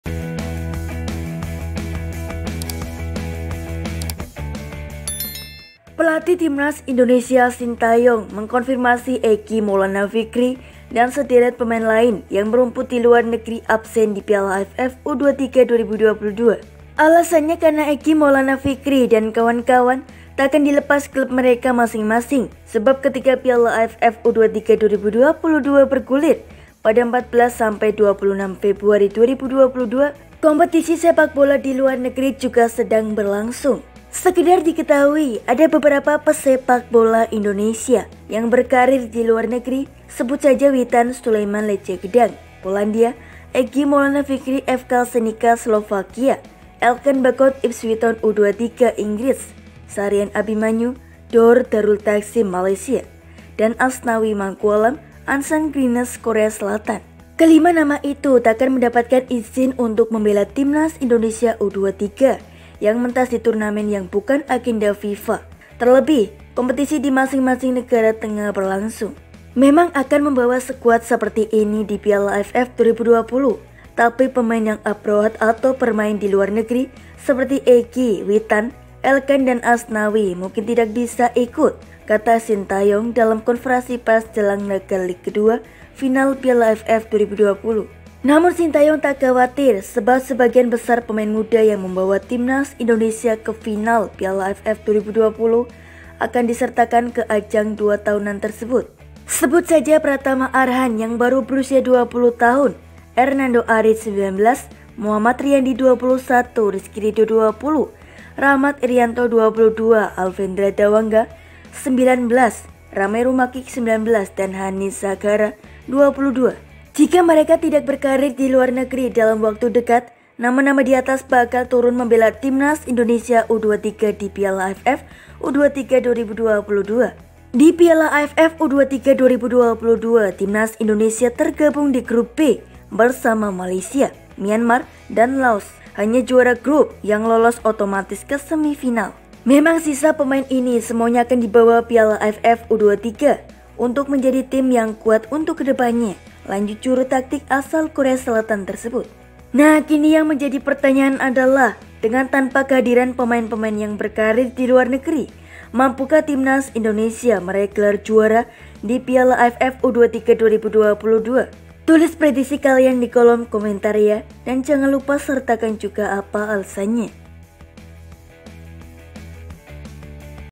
🔴4 PEMAIN TIMNAS INDONESIA DIPASTIKAN TAK MASUK KEDALAM SUSUNAN PEMAIN SKUAD GARUDA DI PIALA AFF U23
Setelah berhasil mengantarkan Timnas Indonesia meraih gelar juara Piala AFF 2020, Pelatih Timnas Indonesia, Shin Tae-yong, mengonfirmasi Egy Maulana Vikri dan sederet pemain lain yang merumput di luar negeri absen di Piala AFF U-23 2022.

Alasannya karena Egy Maulana Vikri dan kawan-kawan takkan dilepas klub mereka masing-masing. Sebab, ketika Piala AFF U-23 2022 bergulir pada 14-26 Februari 2022, kompetisi sepakbola di luar negeri juga sedang berlangsung.

Sekadar diketahui, ada beberapa pesepakbola Indonesia yang berkarier di luar negeri. Sebut saja Witan Sulaeman (Lechia Gdansk, Polandia), Egy Maulana Vikri (FK Senica, Slovakia), Elkan Baggott (Ipswich Town U-23, Inggris), Syahrian Abimanyu (Johor Darul Takzim, Malaysia) dan Asnawi Mangkualam (Ansan Greeners, Korea Selatan).

Kelima nama itu takkan mendapatkan izin untuk membela Timnas Indonesia U-23 yang mentas di turnamen yang bukan agenda FIFA. Terlebih, kompetisi di masing-masing negara tengah berlangsung.

"Memang akan membawa skuad seperti saat ini (di Piala AFF 2020). Tapi pemain yang abroad (bermain di luar negeri) seperti Egy, Witan, Elkan dan Asnawi mungkin tidak bisa ikut," kata Shin Tae-yong dalam konferensi pers jelang laga leg II final Piala AFF 2020.

Namun, Shin Tae-yong tak phawatir. Sebab, sebagian besar pemain muda yang membawa Timnas Indonesia ke final Piala AFF 2020 akan disertakan ke ajang dua tahunan tersebut.

Sebut saja Pratama Arhan yang baru berusia 20 tahun, Ernando Ari (19), Muhammad Riyandi (21), Rizky Ridho (20), Rachmat Irianto (22), Alfeandra Dewangga (19), Ramai Rumakiek (19) dan Hanis Saghara (22).

Jika mereka tidak berkarier di luar negeri dalam waktu dekat, nama-nama di atas bakal turun membela Timnas Indonesia U-23 di Piala AFF U-23 2022.

Di Piala AFF U-23 2022, Timnas Indonesia tergabung di Grup B bersama Malaysia, Myanmar dan Laos. Hanya juara grup yang lolos otomatis ke semifinal.

"Memang sisa pemain ini semuanya akan dibawa (Piala AFF U-23) untuk menjadi tim yang kuat untuk ke depannya," lanjut juru taktik asal Korea Selatan tersebut.

Nah kini yang menjadi pertanyaan adalah, dengan tanpa kehadiran pemain-pemain yang berkarier di luar negeri, mampukah Timnas Indonesia meraih gelar juara Piala AFF U-23 2022?

Tulis prediksi kalian di kolom komentar ya, dan jangan lupa sertakan juga apa alasanya.